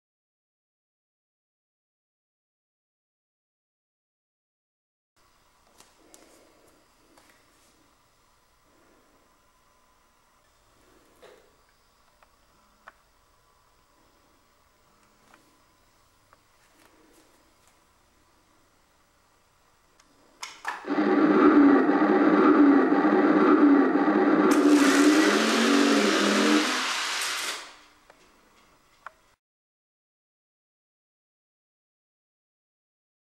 Nissan Skyline GTR vs Monte Carlo SS
Monte Carlo Loses control, wobbles and wins